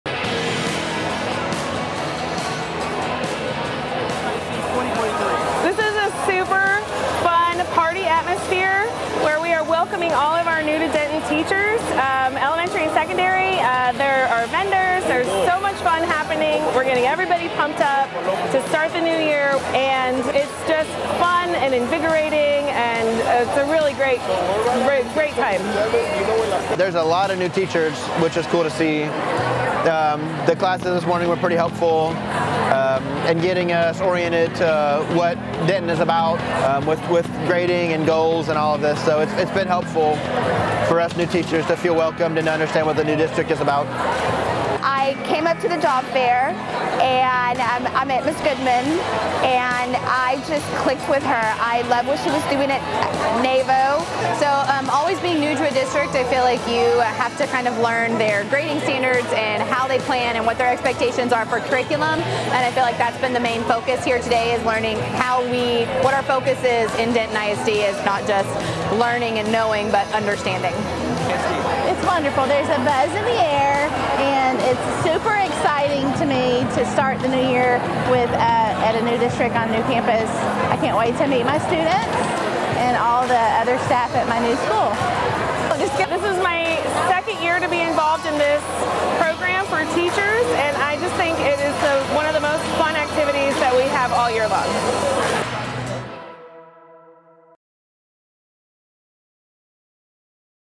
DISD New Hire Event 2023
Our new teachers are welcomed to the district with a day of learning both inside and outside of the classroom.